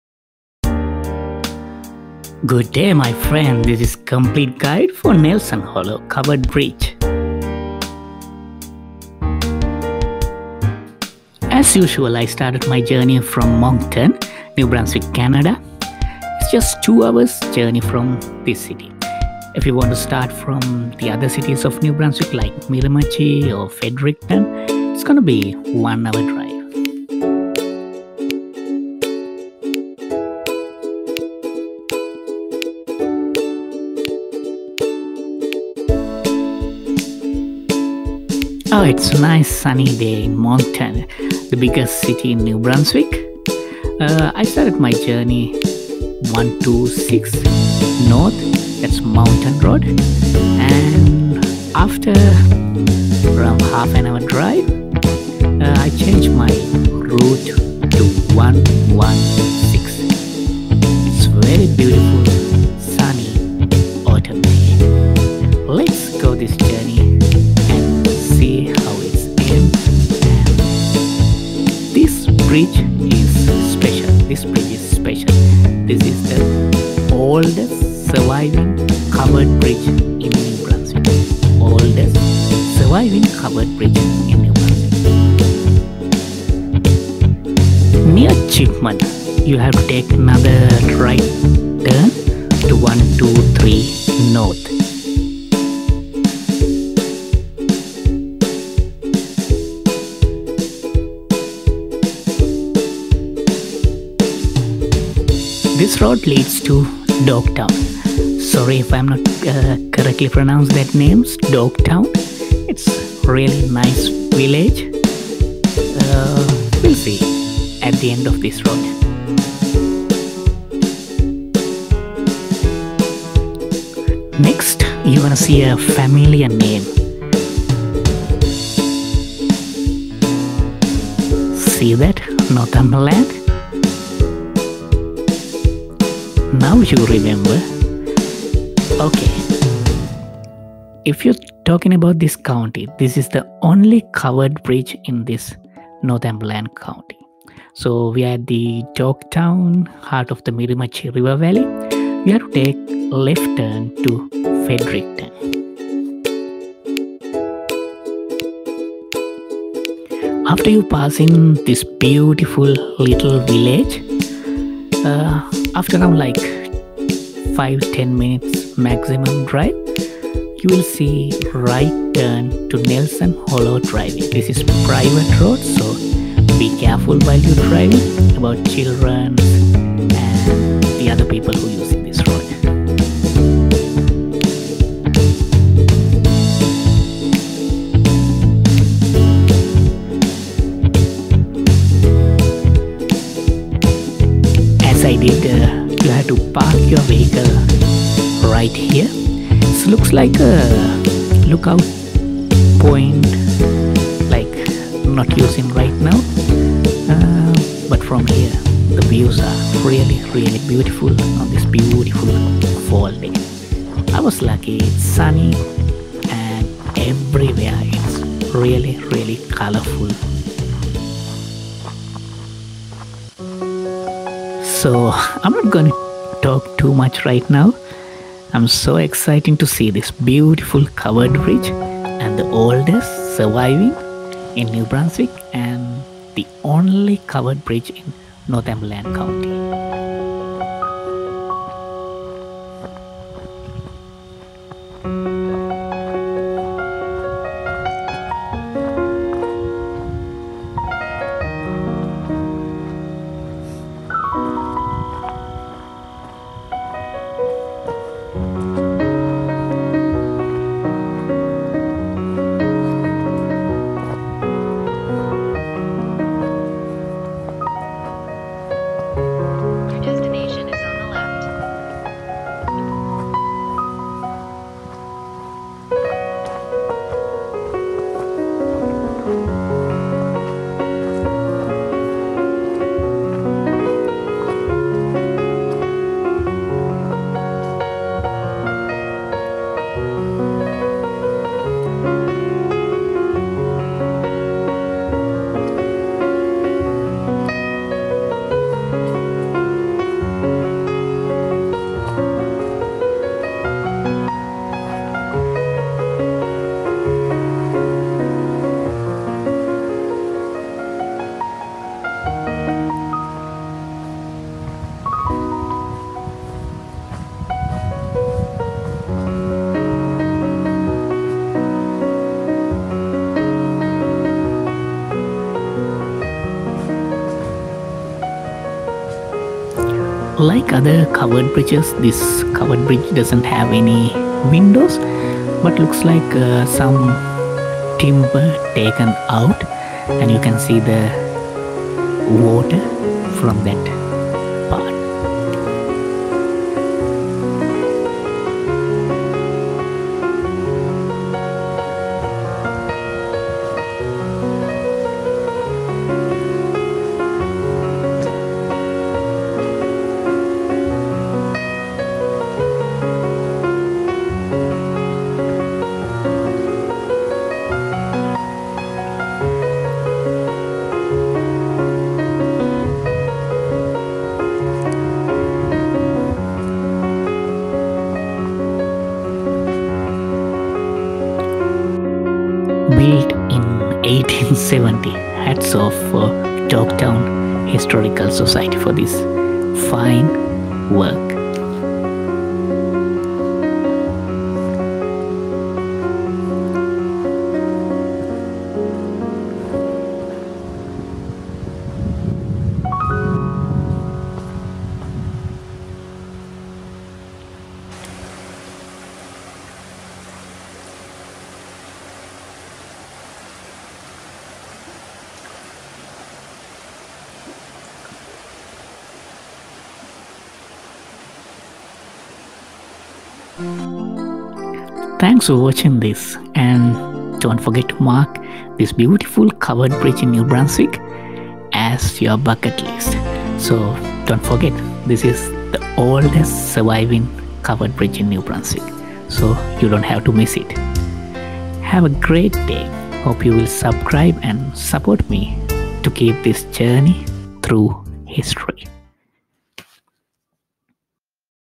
Nelson Hollow Covered Bridge
Watch HD. This is My 5th Video about Covered Bridges. The Oldest surviving Covered Bridge.

And the Only Covered Bridge in Northumberland

Country: Canada
Province: New Brunswick
County: Northumberland

MILL BROOK #0.5 (Nelson Hollow)
Route 8, Exit at Nelson Hollow Drive or Lyons Road. (Not in Service).

Length 24.5m (80' 6").